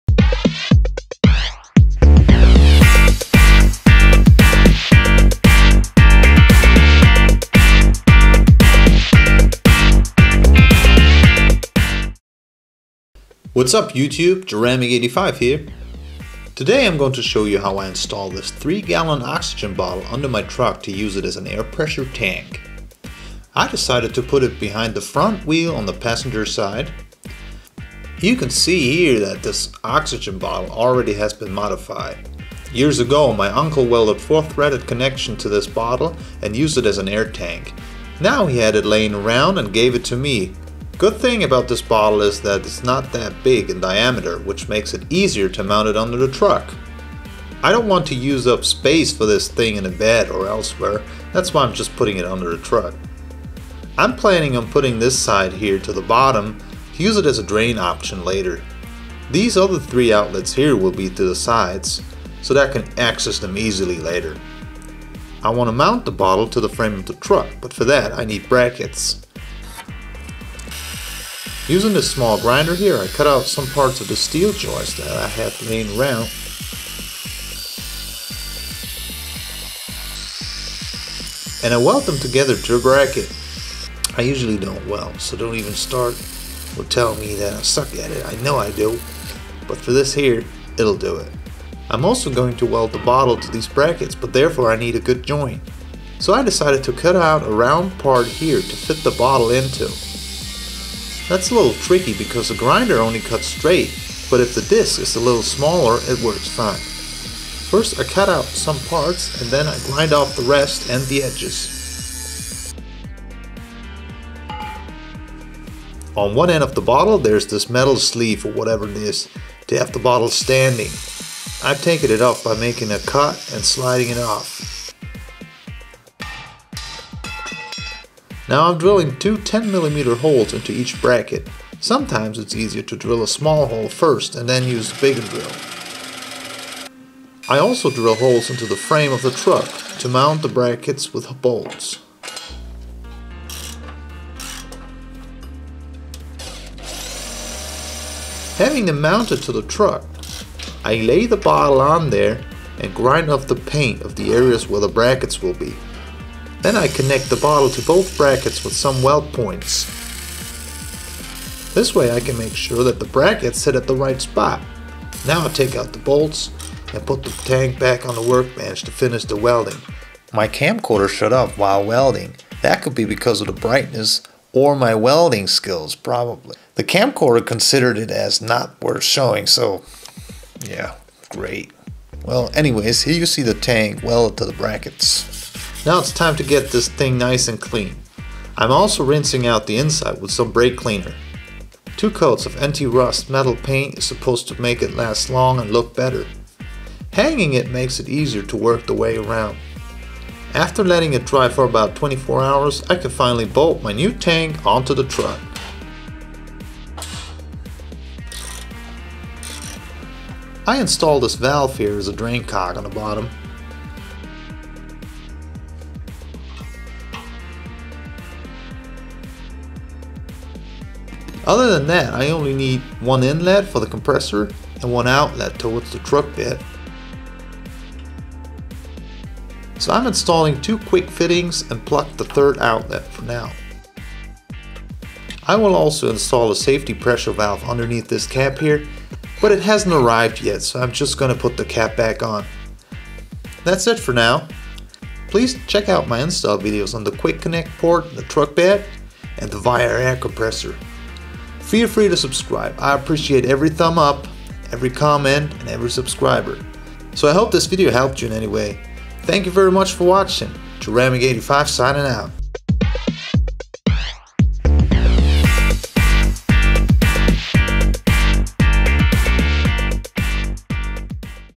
HOW TO BUILD CUSTOM AIR TANK+CAR INSTALL STORAGE BOTTLE ONBOARD | PICK UP TRUCK VIAIR COMPRESSOR KIT
in this video i show you how i build my own air pressure tank and install it on my dodge ram. thanks for watching. check out my channel for more videos on onboard air system.